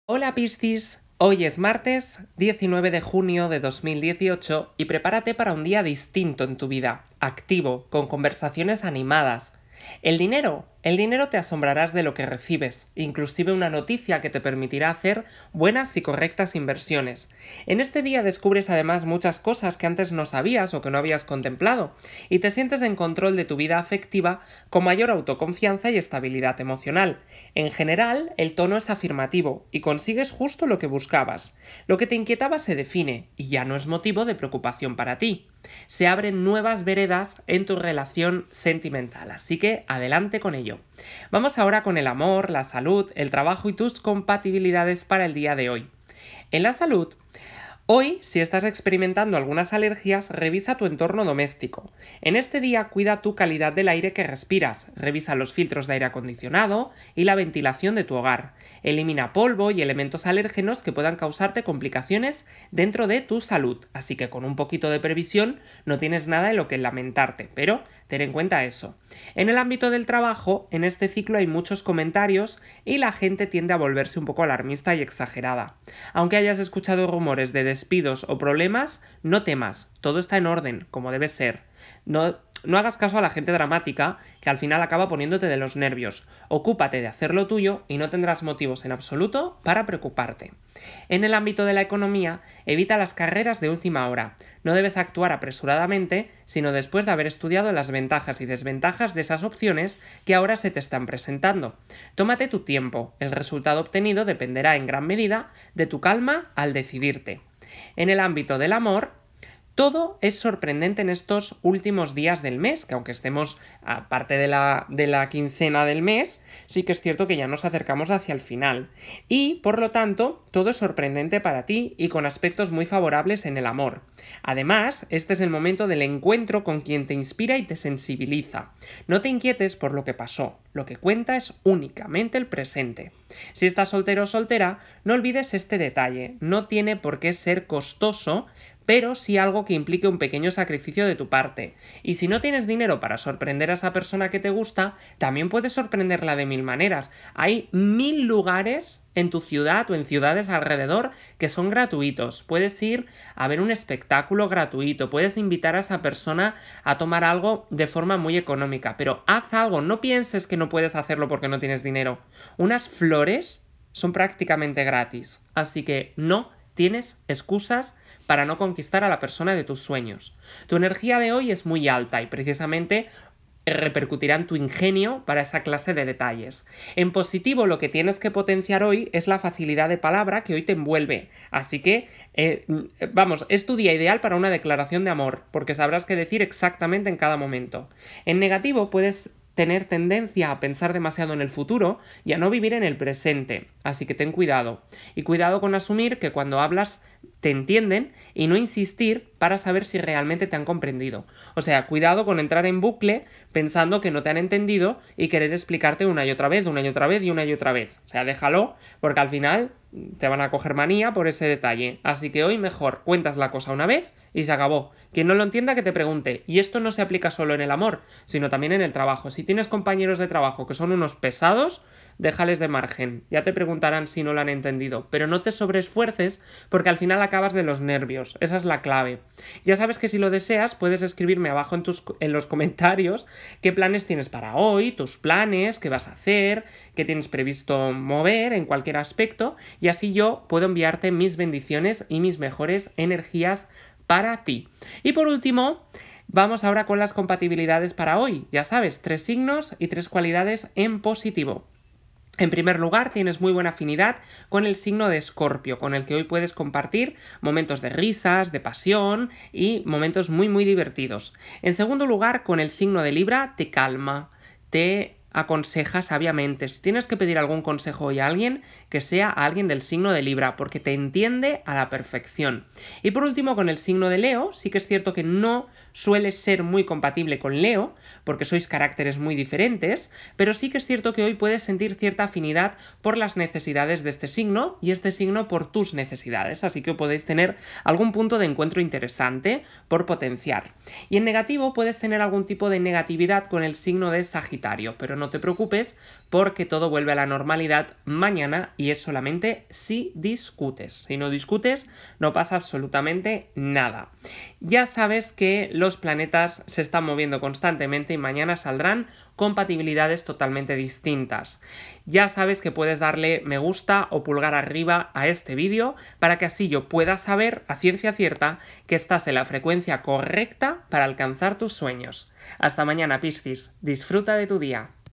Horóscopo Diario - Piscis - 19 de Junio de 2018
Horóscopo de hoy Piscis 19 de Junio de 2018. Horóscopo gratis.
Descubre tu futuro con mi horóscopo diario para ti, mi querido signo Piscis en el amor, el trabajo, la salud, el dinero y las compatibilidades con otros signos para hoy.